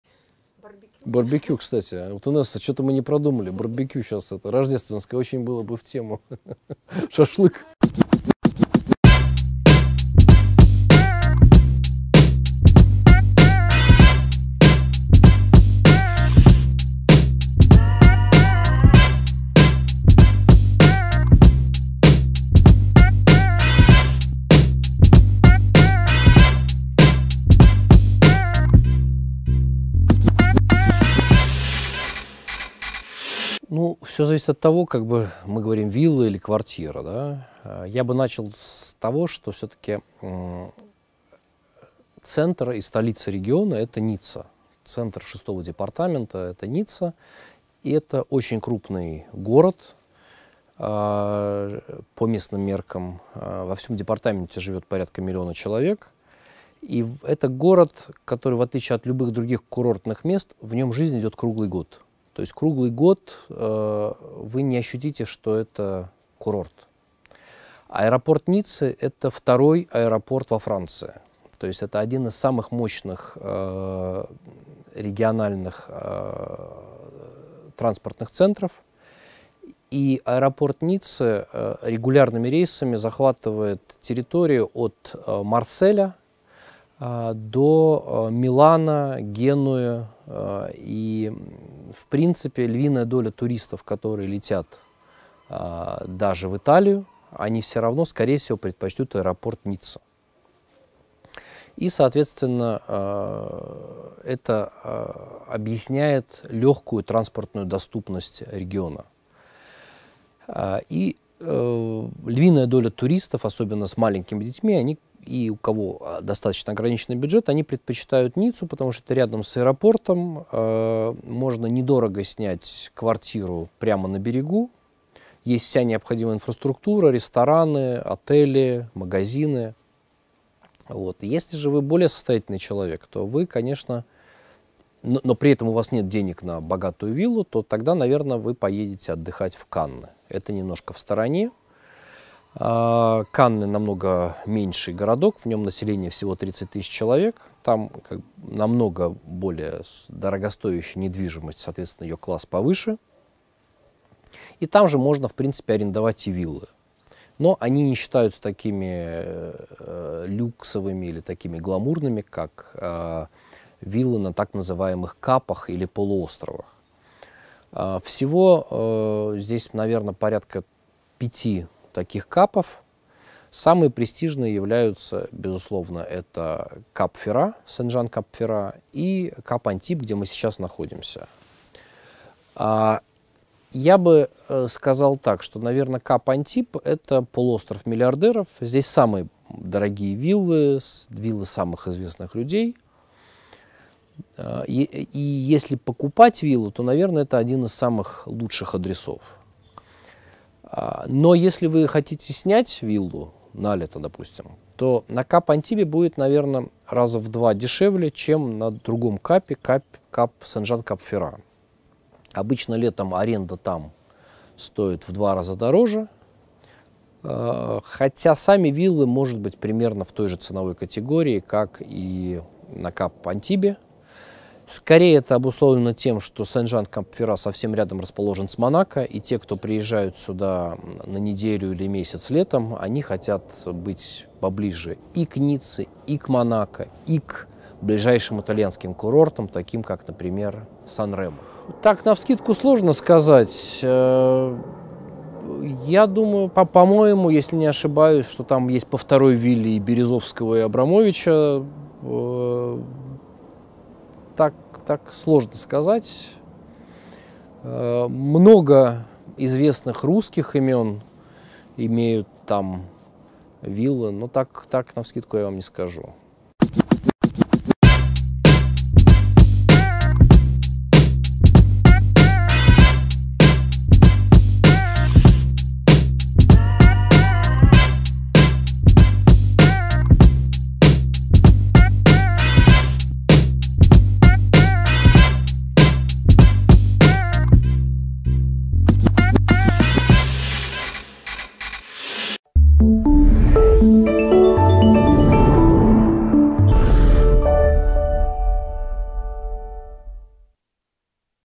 Жить в Ницце. Про Лазурный берег. Из интервью с Виталием Архангельским
Узнайте про Лазурный Берег много нового и интересного. Где предпочитают жить олигархи, где лучше покупать себе жилье или арендовать его, особенности проживания на Лазурном Берегу.